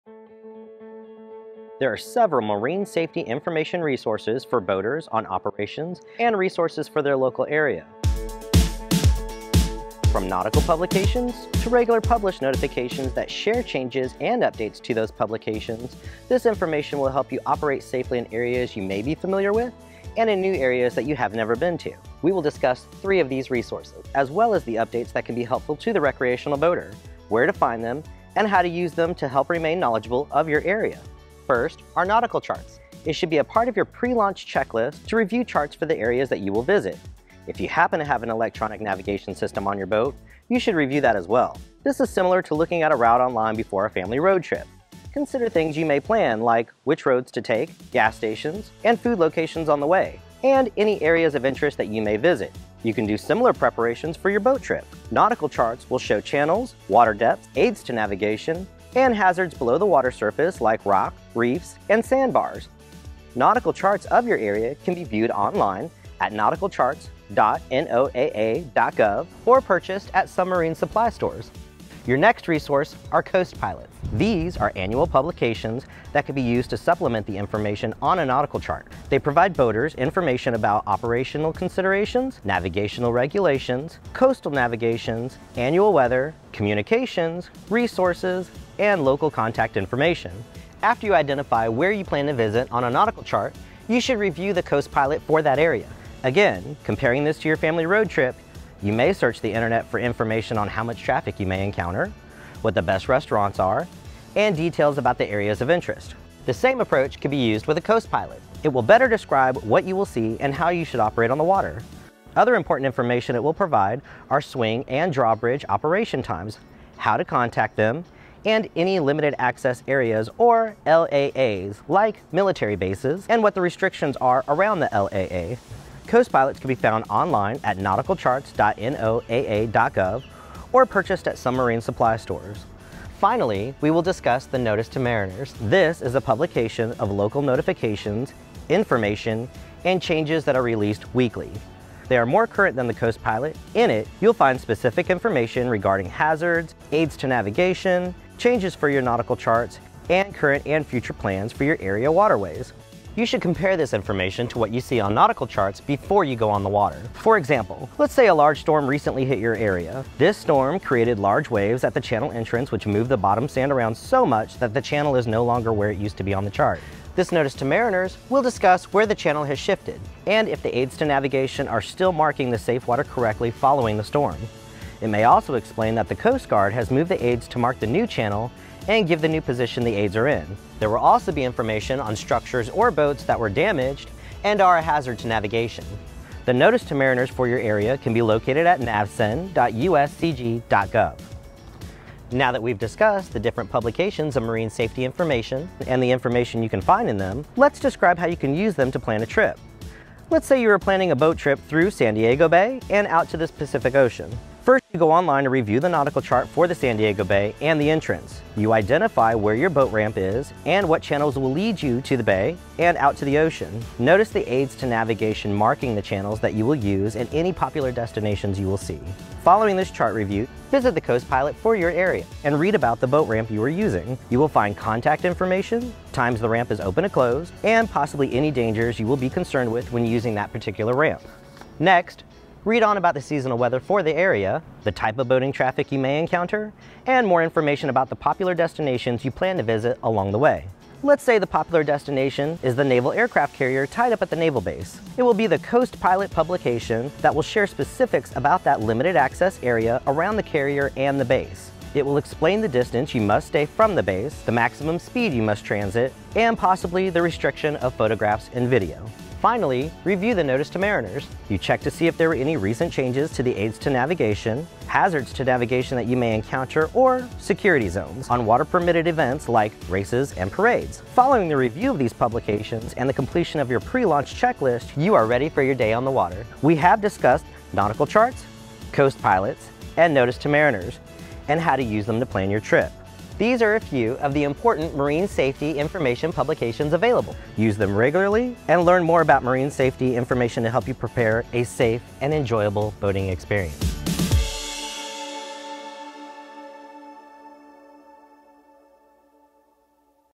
BoatOnCourse.com: Marine Safety Information
Boat On Course, educational boating safety videos from the National Safe Boating Council, teach the basic navigation rules of boating. In this video, boaters will learn about several marine safety information resources for boaters on operations and resources for their local area. From nautical publications to regular published notifications that share changes and updates to those publications, this information will help you operate safely in areas you may be familiar with and in new areas that you have never been to.

Boat On Course is produced under a grant from the Sport Fish Restoration and Boating Trust Fund, administered by the U.S. Coast Guard.